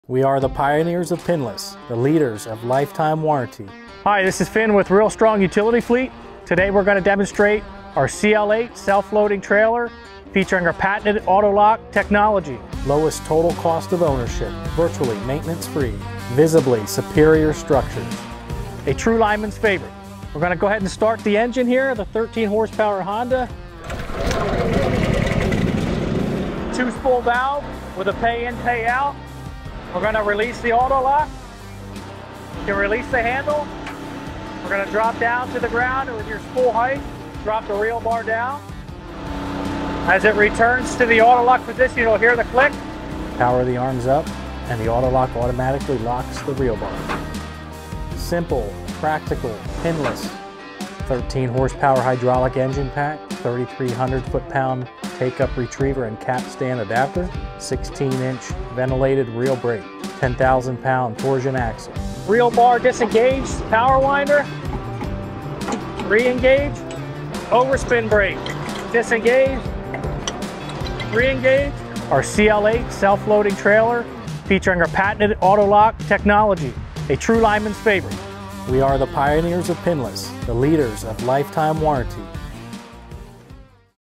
REELSTRONG Utility Fleet | CL 8 Pinless Reel Trailer
REELSTRONG Pinless Reel Trailer
The CL 8 AUTOLOCK™ is a true linemen favorite. Just back up to the reel, power the arms up and the LIFETIME WARRANTY latch automatically locks the reel bar for the safest, virtually maintenance free and effortless self-loader on the market.